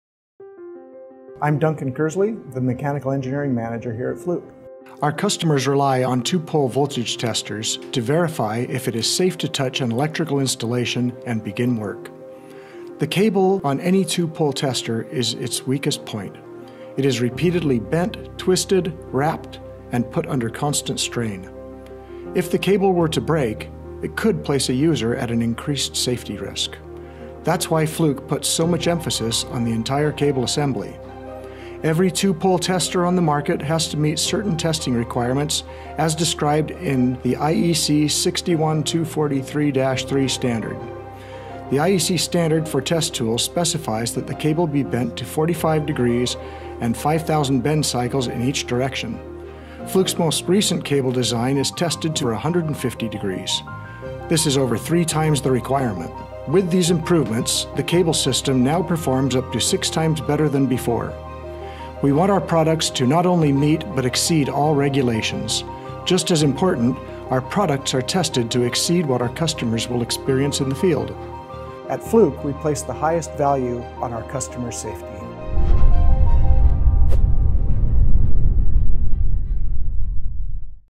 Wie testet Fluke die neuen und verbesserten zweipoligen Spannungsprüfer?
Das Kabel jedes zweipoligen Spannungsprüfgerätes ist der schwächste Punkt. Aus diesem Grund wurden die zweipoligen Spannungsprüfer von Fluke über die normalen Sicherheitsstandards hinaus getestet, um die Sicherheit unserer Kunden zu gewährleisten.

In diesem Video erfahren Sie, wie Fluke die neuen und verbesserten zweipoligen Spannungsprüfer getestet hat.